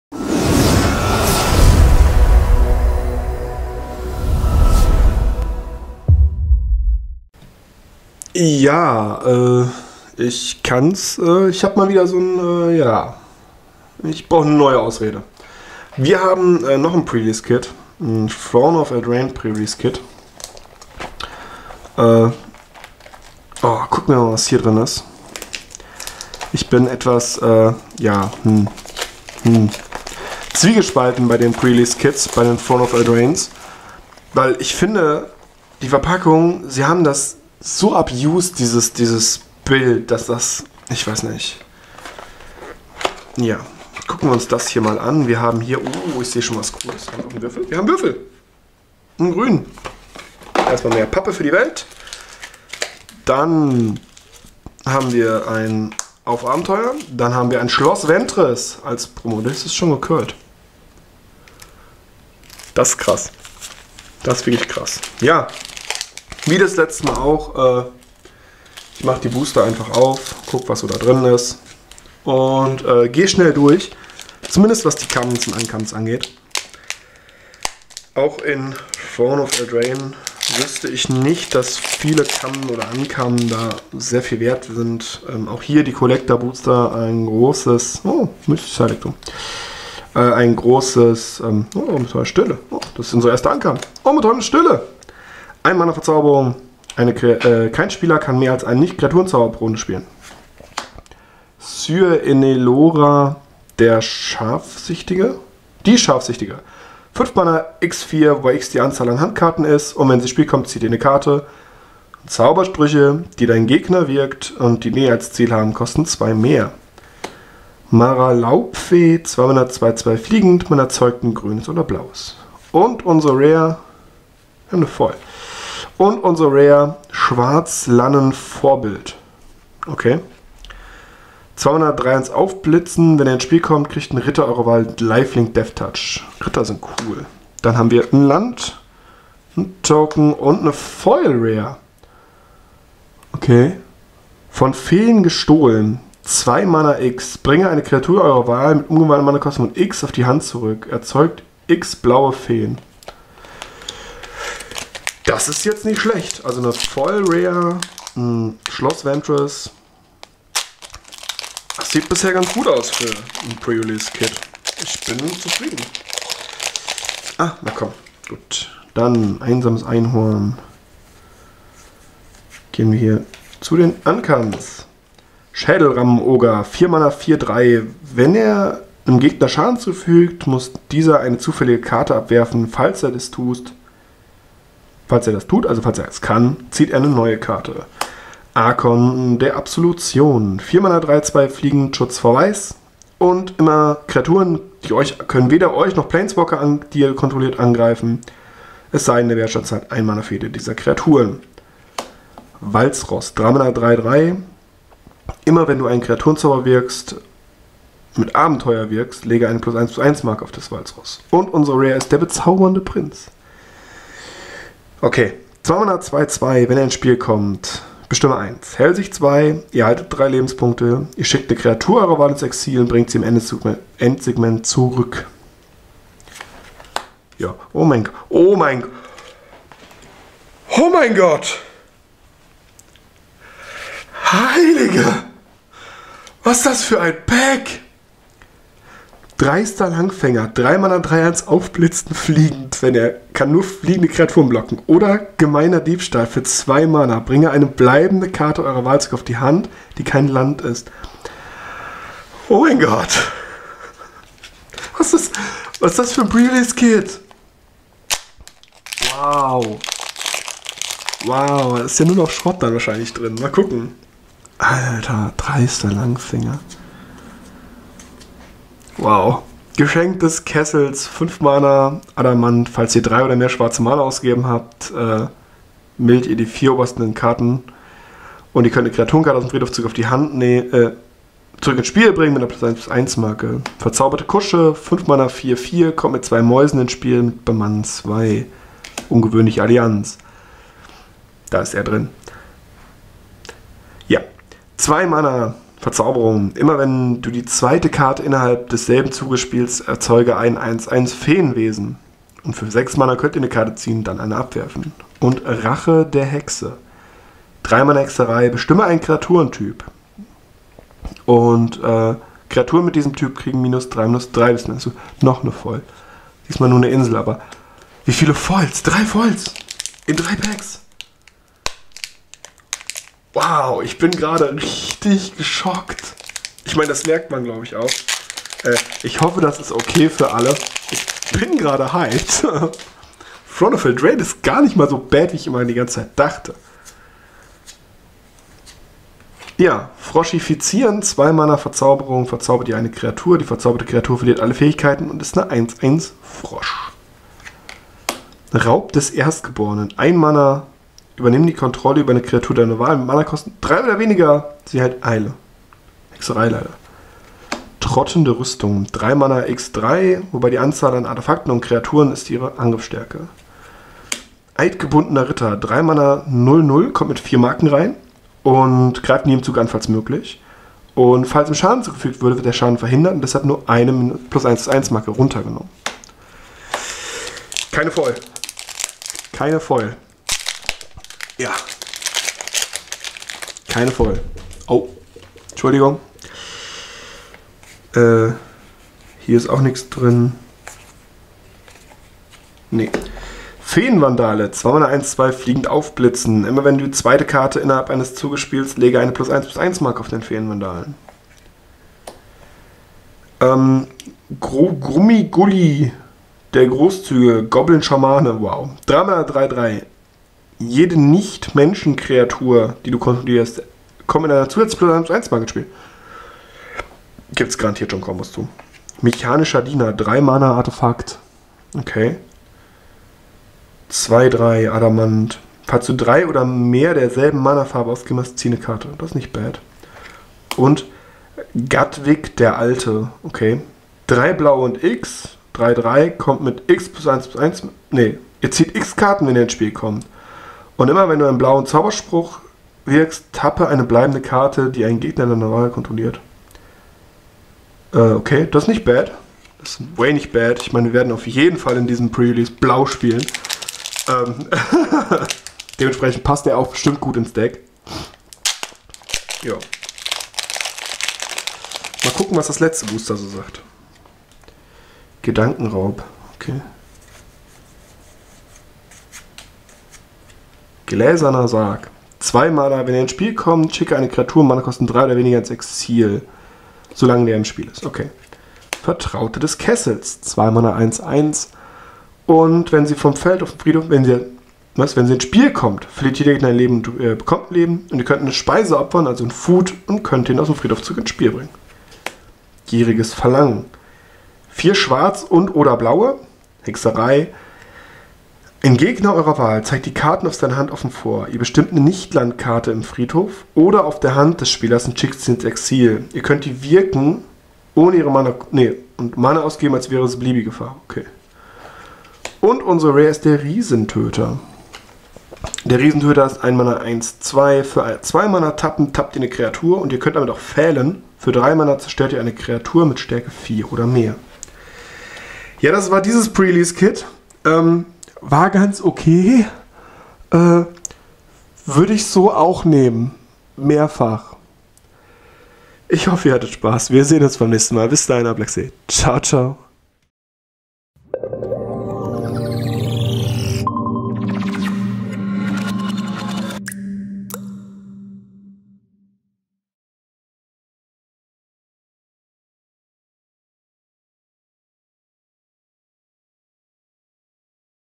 Throne of Eldraine PreReleaseKit Opening
Ich hab mal ein weiteres PreReleaseKit gekauft, warum tu ich das? Throne of Eldraine hat uns aber immer gut behandelt. Wird es das diesesmal auch tun?